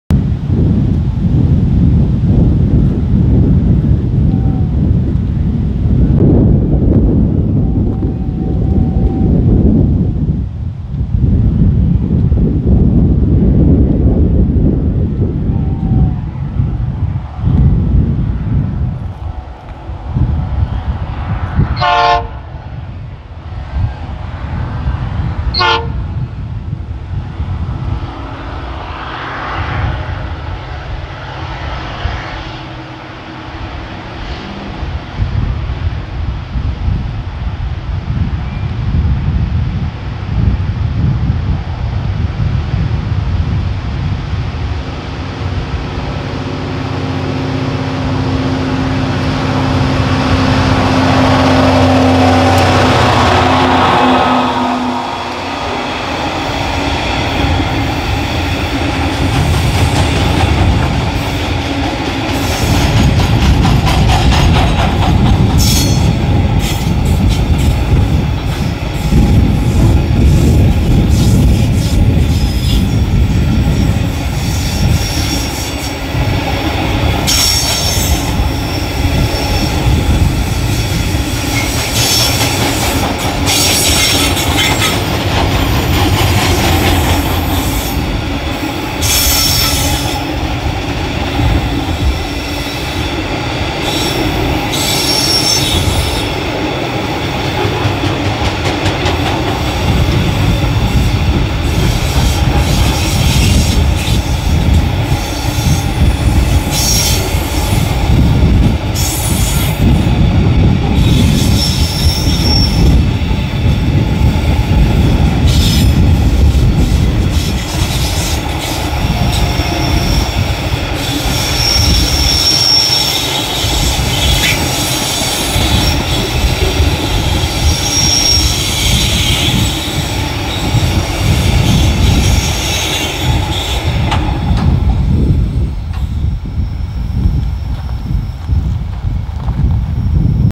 Trains in Western Australia: 6974 North Dandalup
Filmed on the 1st of January, 2021.
S3305 leads 6974, a loaded loaded Bauxite train seen at North Dandalup.